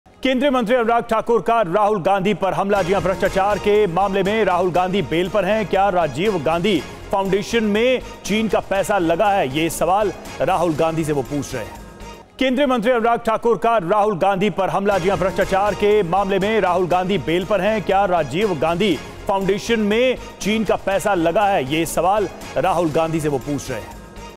भ्रष्टाचार के मामले में राहुल गांधी बेल पर हैं- Anurag Thakur | Bjp On Rahul Gandhi | Congress
केंद्रीय मंत्री अनुराग ठाकुर का राहुल गांधी पर हमला, कहा- भ्रष्टाचार के मामले में राहुल गांधी बेल पर हैं, क्या राजीव गांधी फाउंडेशन में चीन का पैसा लगा है.